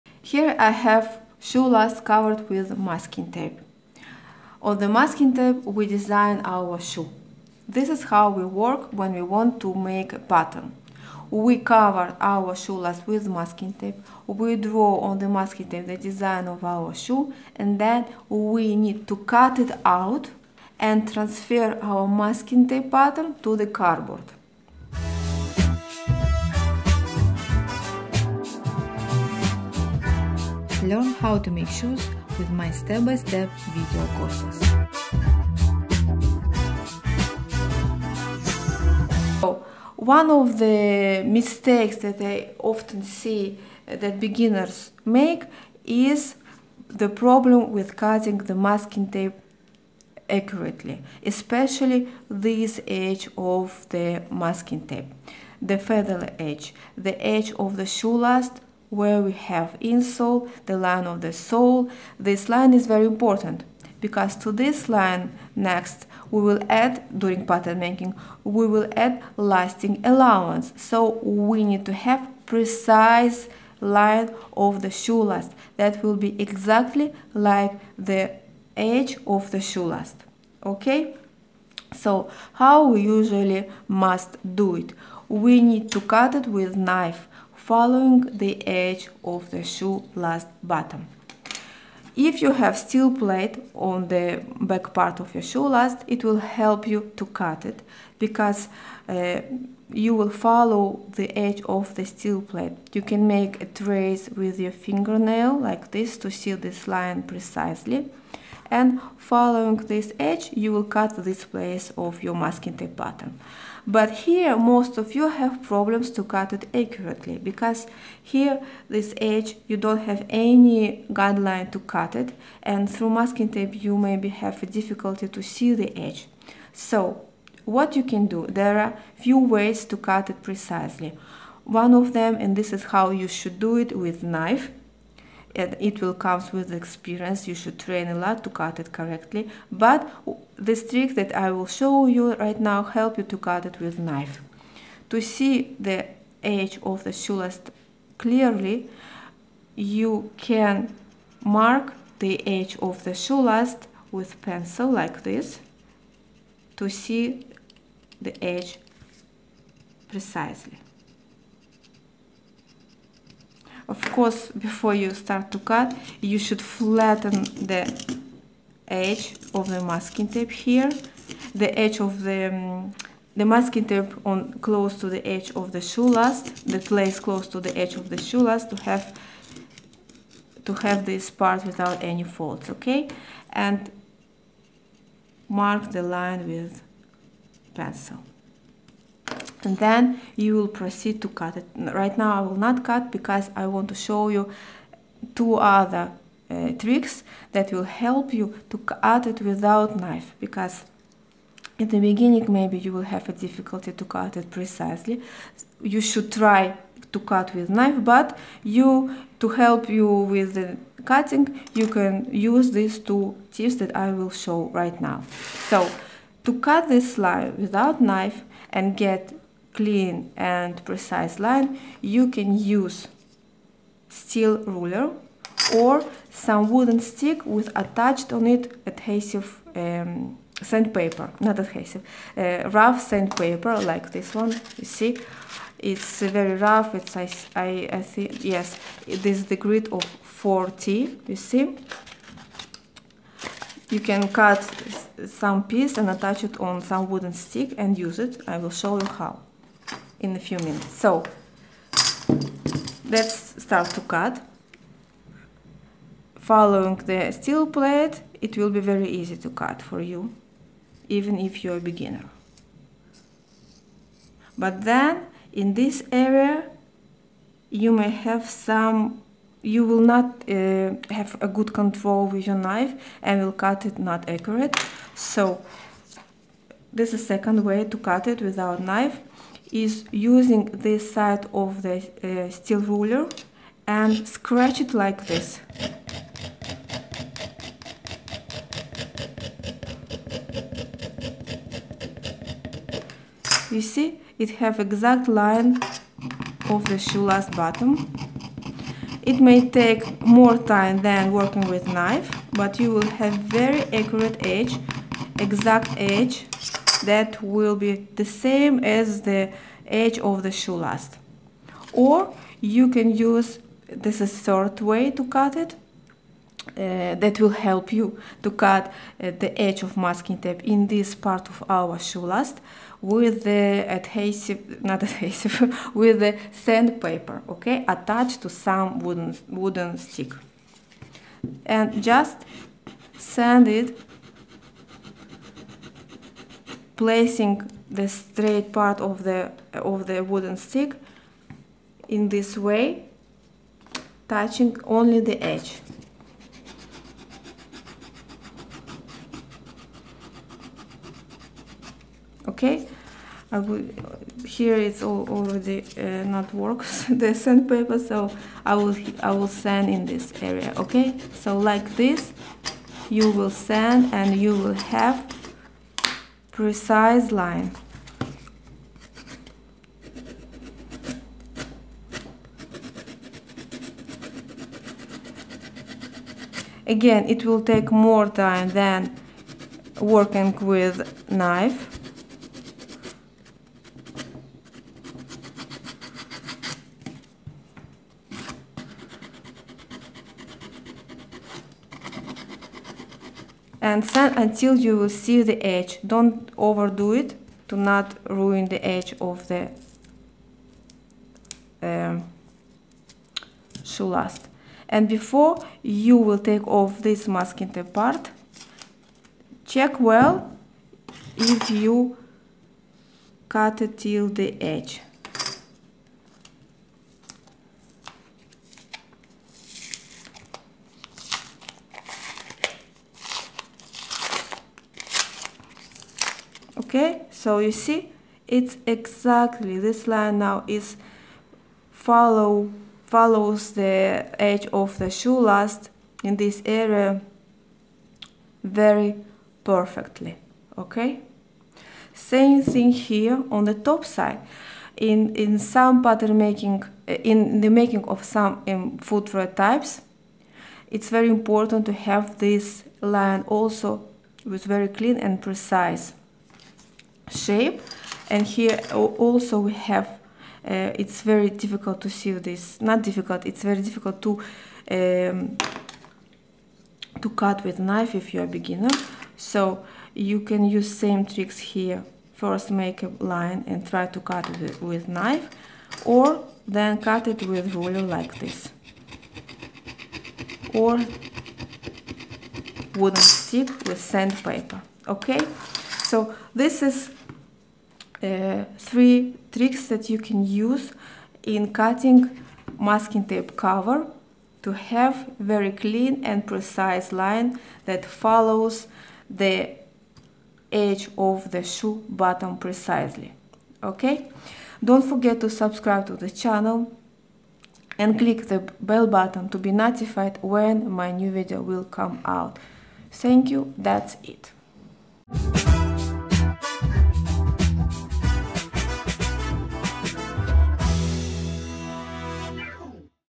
How to Cut Masking tape Pattern like a Pro [Shoemaking tips]
These are your first steps in making shoes, and if you are a beginner in shoemaking you should learn how to do it right.
Attaching a masking tape correctly on a shoe last is a skill that you need to develop but the skill of taking this masking tape pattern off the shoe last can be a challenge as well.
I see many beginners struggle with that and ruin their beautiful masking patterns just because they are not skilled with the knife.
So in today's video, I will teach you 3 ways how you, beginners can cut these masking tape patterns clean.